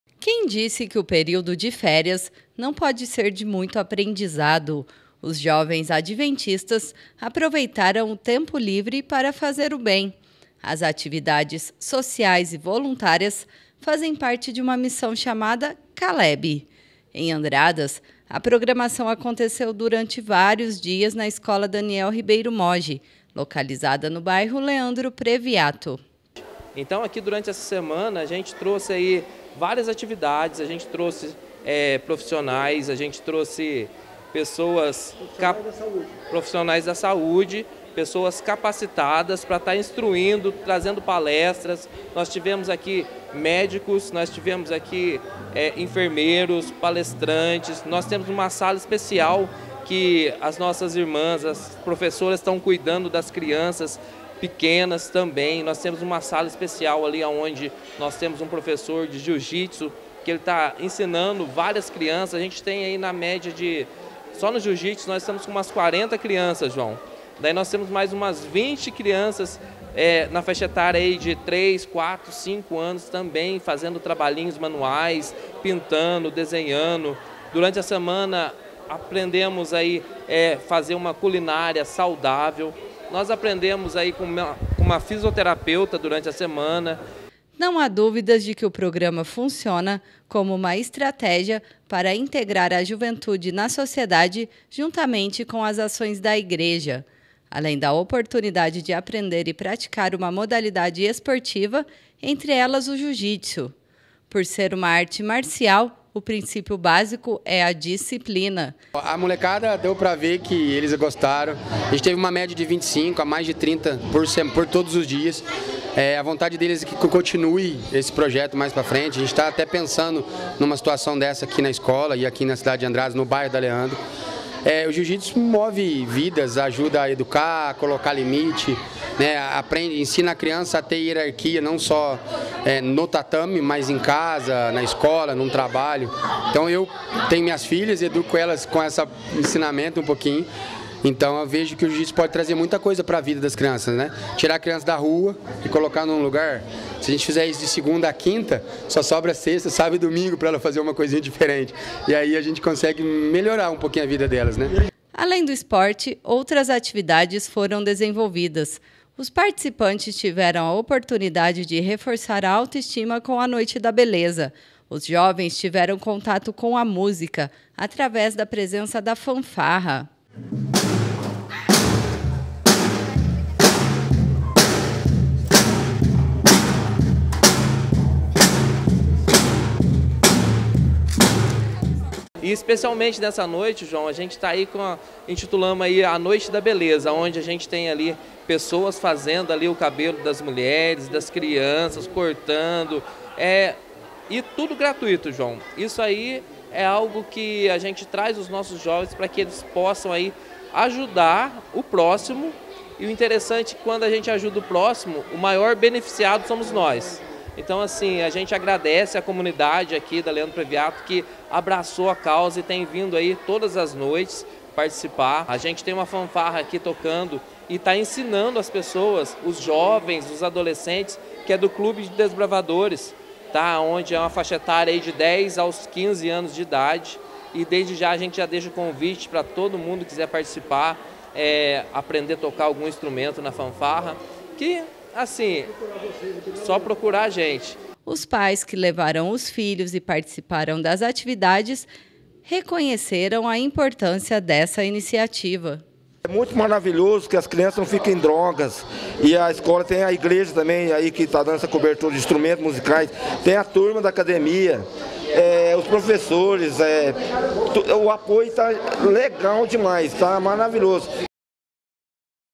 Jovens Adventistas desenvolvem ações sociais durante as férias
Durante o mês de janeiro adventistas desenvolveram a missão Calebe. O programa voluntário que desafia os jovens a dedicarem o tempo das férias ao evangelismo para ações sociais e novas experiências.